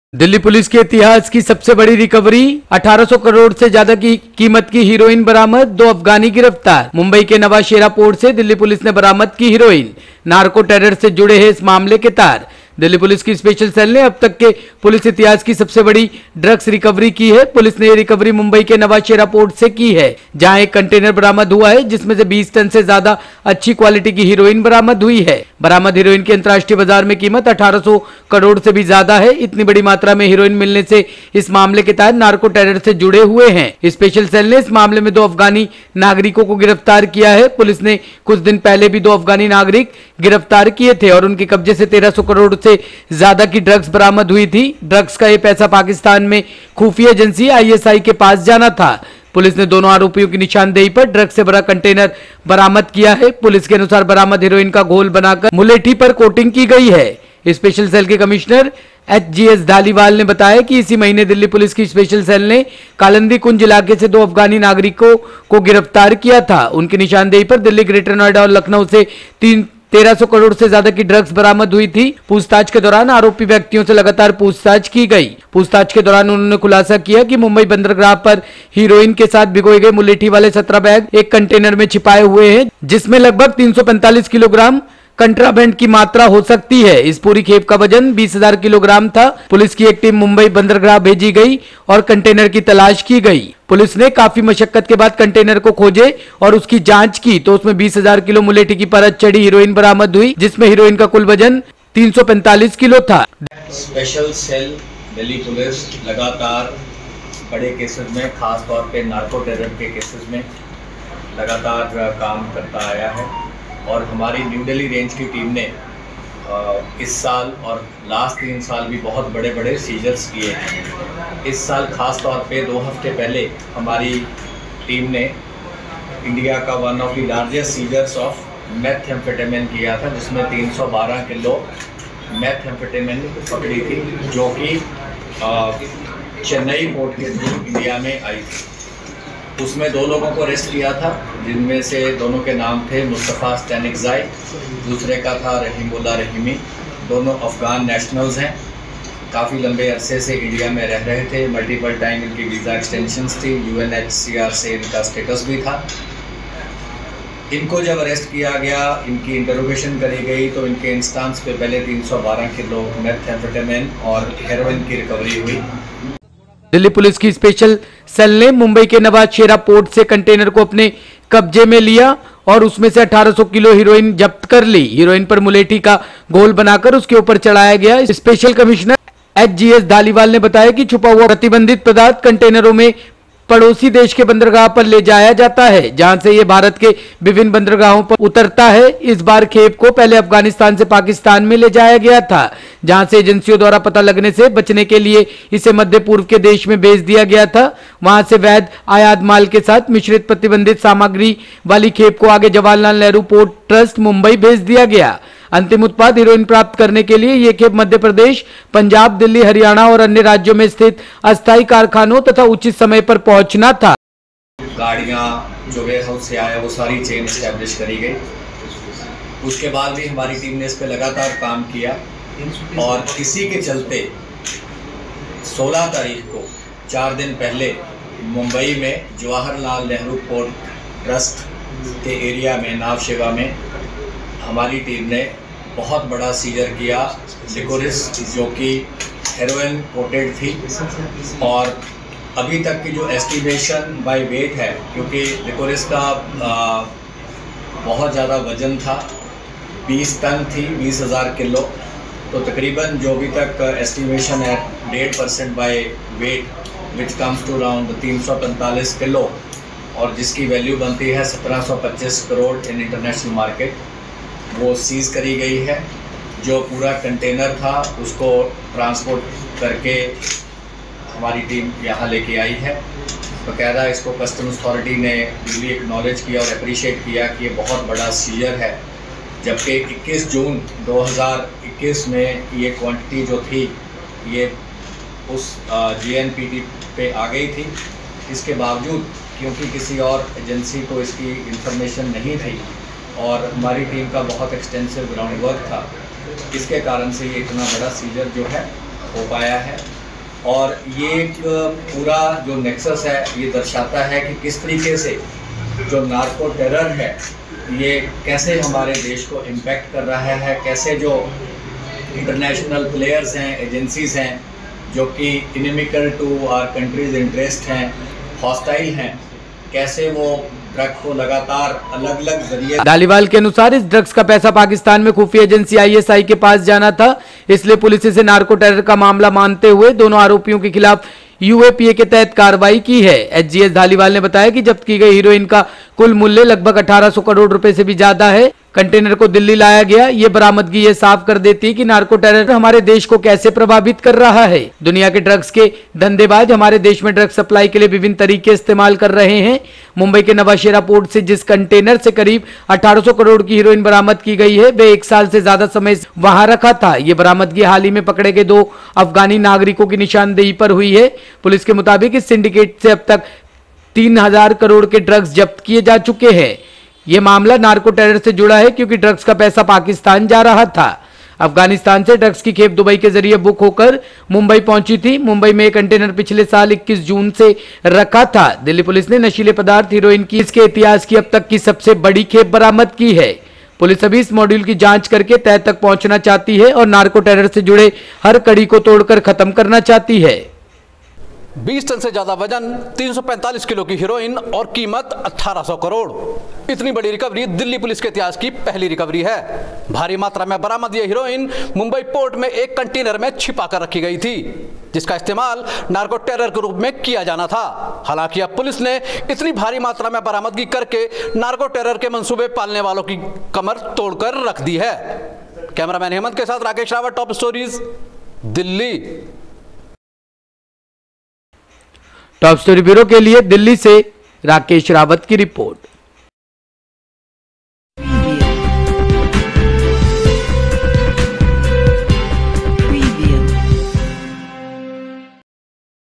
दिल्ली पुलिस की बड़ी रिकवरी,1800 करोड़ से ज्यादा कीमत की हेरोइन बरामद, दो अफगानी गिरफ्तार
दिल्ली पुलिस की स्पेशल सेल ने अब तक के पुलिस इतिहास की सबसे बड़ी ड्रग्स रिकवरी की है। पुलिस ने ये रिकवरी मुंबई के नवा शेरा पोर्ट से की है जहां एक कंटेनर बरामद हुआ जिसमे से 20 टन से ज्यादा अच्छी क्वालिटी की हेरोइन बरामद हुई। बरामद हेरोइन की अंतराष्ट्रीय बाजार में कीमत 1800 करोड़ से भी ज्यादा है। इतनी बड़ी मात्रा में हेरोइन मिलने के इस मामले के तार नार्को टेरर से जुड़े हुए है। स्पेशल सेल ने इस मामले में दो अफगानी नागरिको को गिरफ्तार किया है। पुलिस ने कुछ दिन पहले भी दो अफगानी नागरिक गिरफ्तार किए थे और उनके कब्जे से भी 1300 करोड़ से ज्यादा की ड्रग्स बरामद हुई थी। ड्रग्स का ये पैसा पाकिस्तान में खुफिया एजेंसी आईएसआई के पास जाना था। पुलिस ने दोनों आरोपियों की निशानदेही पर ड्रग्स से भरा कंटेनर बरामद किया है। पुलिस के अनुसार बरामद हेरोइन  का घोल बनाकर मुलेठी पर कोटिंग की गई है